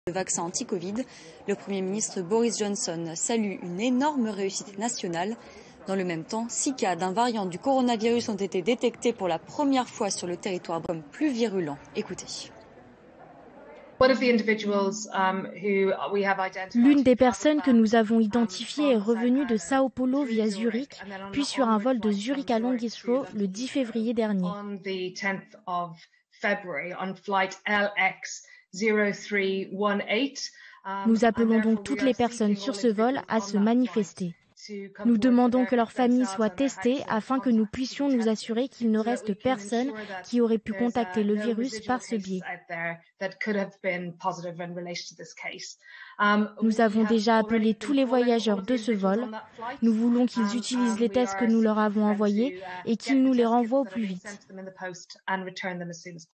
شناسایی نوع برزیلی و کشنده کرونا در انگلیس
مقامات بهداشتی انگلیس اعلام کردند شش مورد آلوده به سویه برزیلی ویروس کووید-۱۹ در انگلیس مشاهده شده است که در مقایسه با ویروس اصلی، کشنده‌تر است .